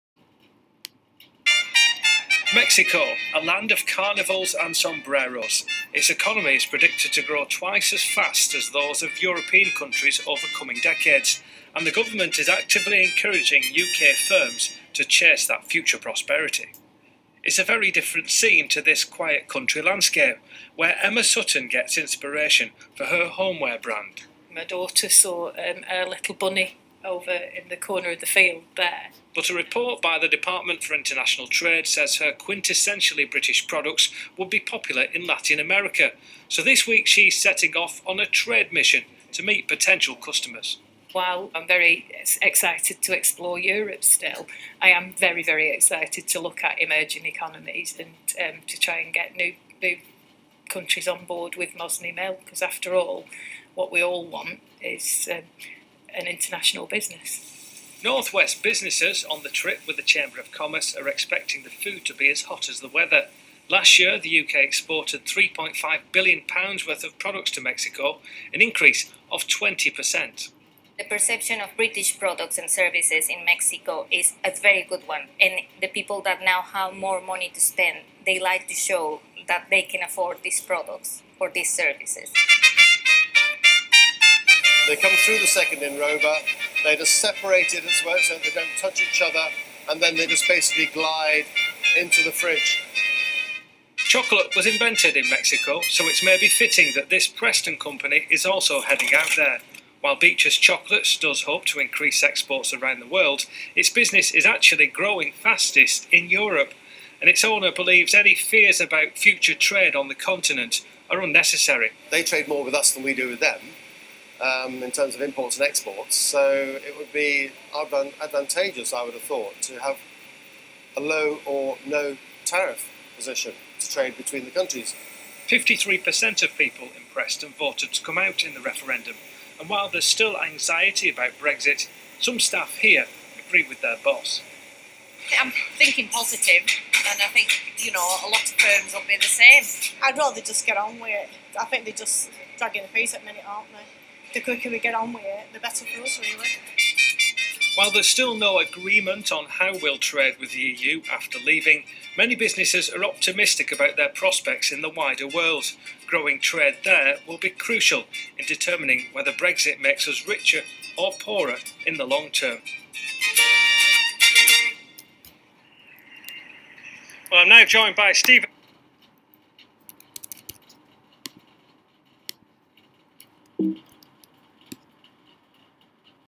BBC reports BritChaM's Trade Mission
On the 19th March the UK watched how some companies are taking the leap over the Atlantic to test their products in Mexico. The British Chamber of Commerce Mexico are welcoming Mosney Mill and Beech's Fine Chocolates, as well as other British companies, on our trade mission at the end of this month.